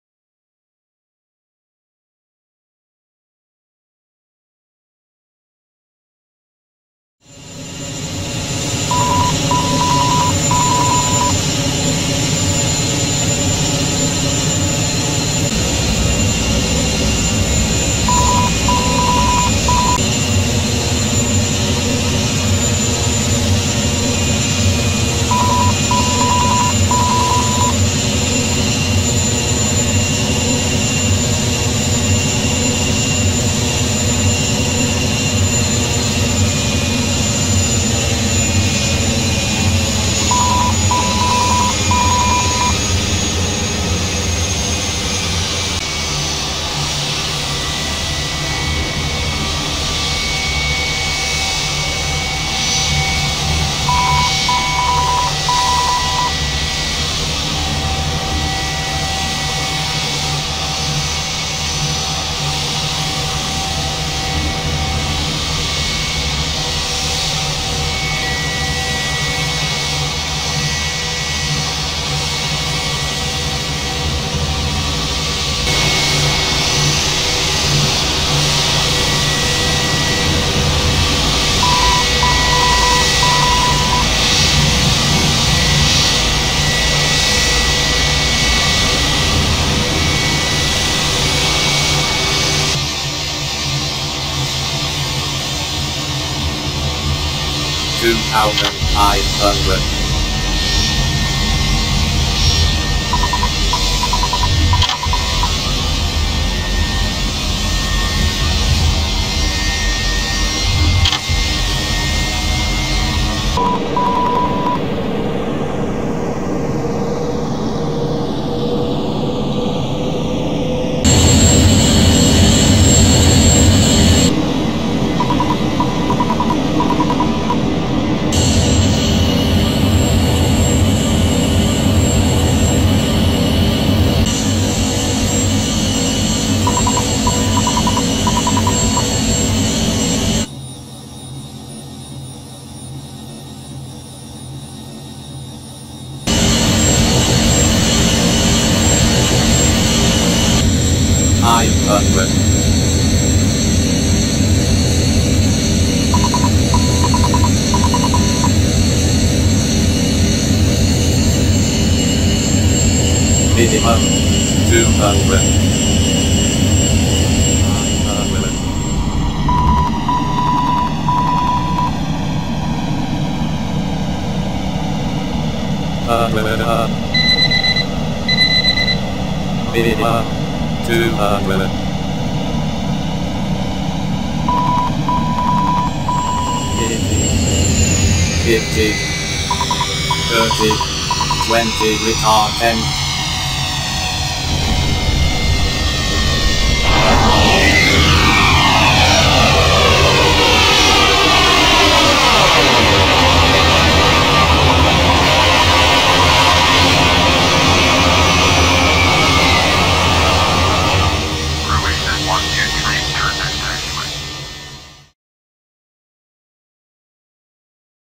Multi views landing Airport Dubrovnik in Croatia.
Have you ever been in Dubrovnik. Arriving with the plane from the north is giving a beautifull view of the town and the city from the Roman time.
The approach is wonderfull and filmed from multi views. Enjoy the landing.